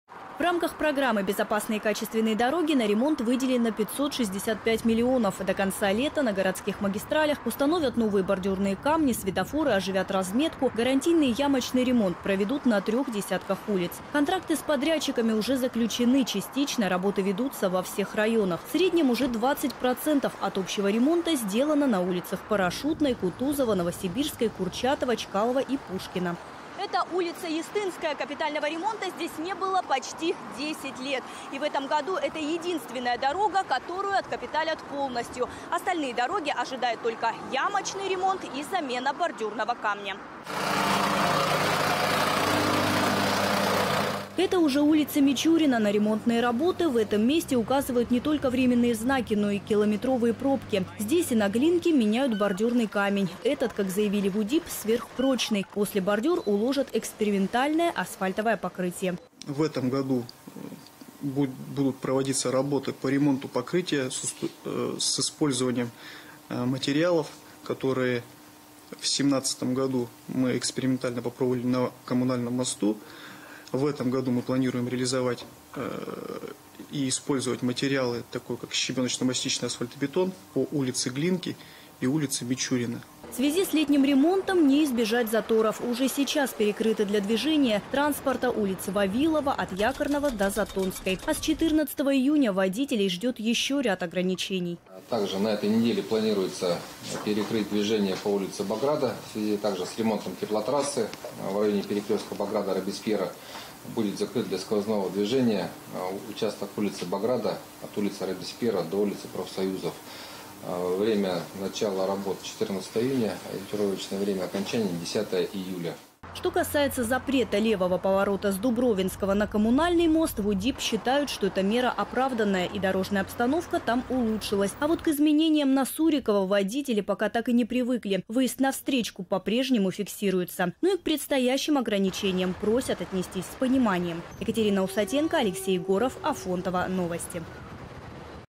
Ремонт дорог в Красноярске
До конца августа рабочие должны отремонтировать 15 городских магистралей.

На это потратят более 500 миллионов рублей. Сумма вдвое меньше, чем в прошлом году.

Екатерина Усатенко выясняла, где ремонт уже начался и какие улицы в ближайшее время будут недоступны.